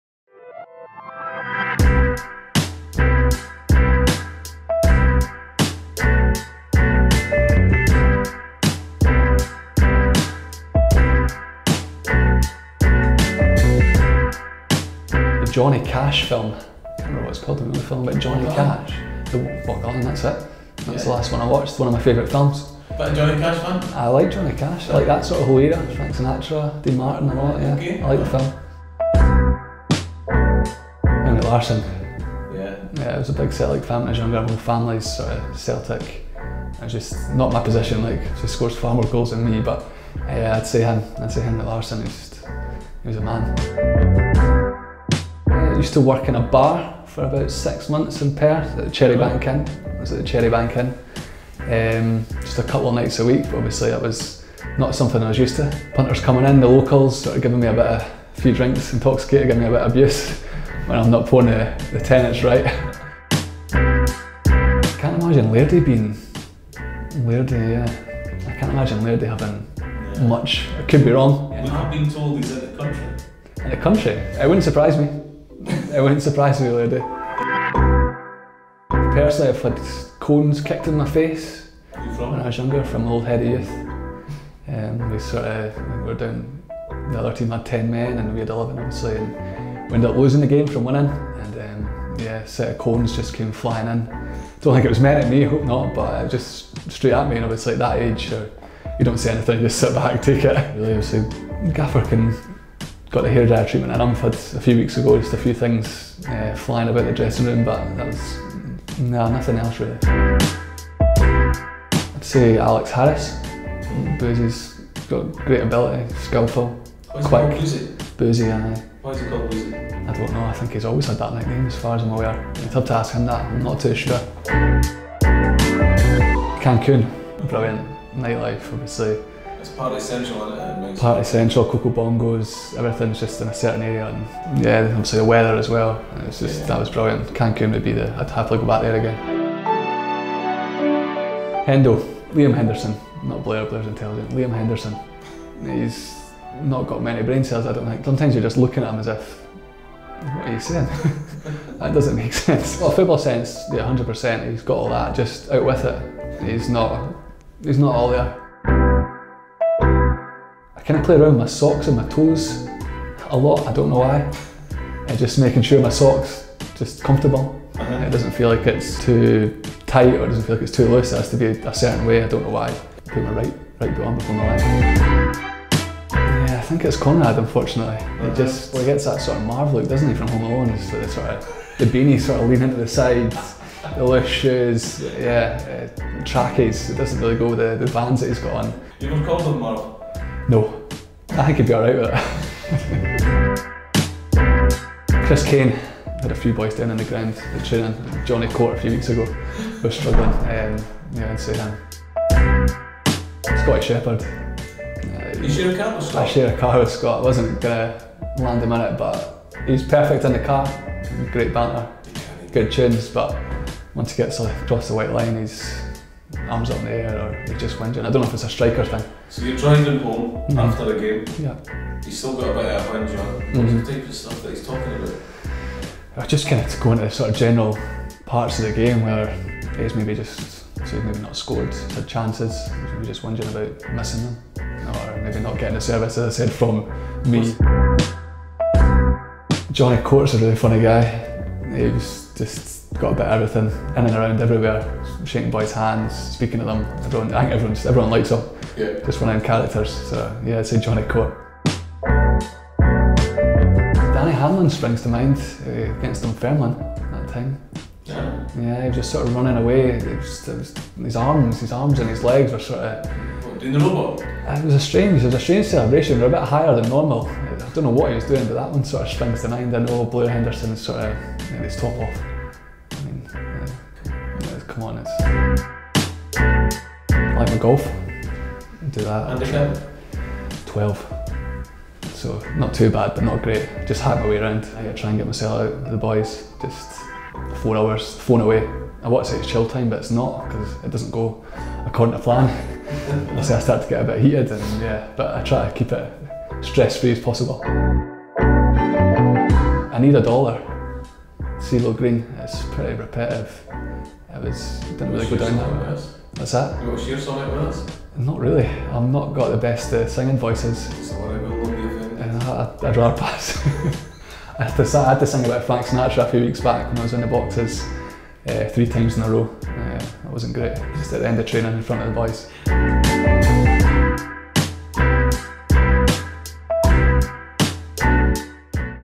On the Bench | Kieran Stewart
Kieran Stewart takes his seat on the bench to talk about his interests and his teammates.

Filmed by @JallanFilms